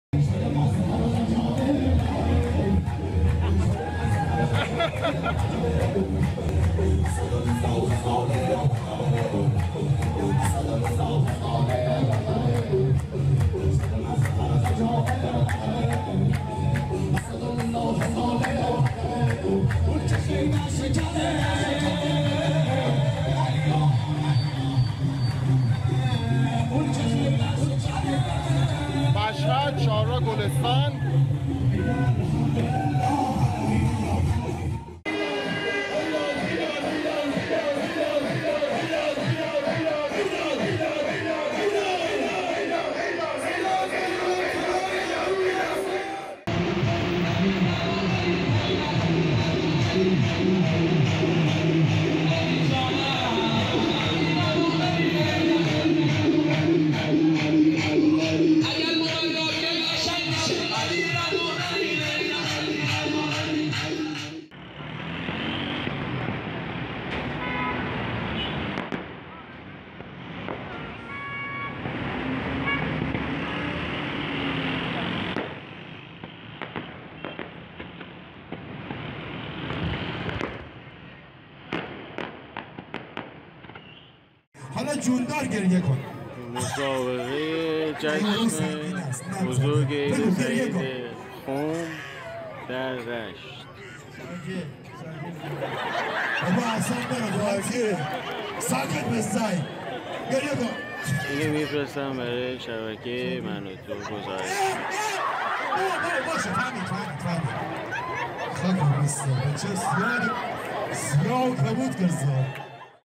مراسم جشن و کارناوال‌های شادی عید غدیر در شهرهای مختلف ایران
دوشنبه شب (۲۹ شهریور ماه ۹۵)، در مناطق مختلف ایران، به مناسبت عید غدیر، خیابان‌ها را  آذین‌بندی کرده بودند و آتش‌بازی‌ها و نورافشانی‌های بزرگ و جشن‌هایی برپا کرده بودند.
همچنین به همین مناسبت در خیابان‌ها ایستگاه‌های شادمانی برپا شده بود و گروه‌هایی هم مشغول پخش شربت و شیرینی بین مردم بودند.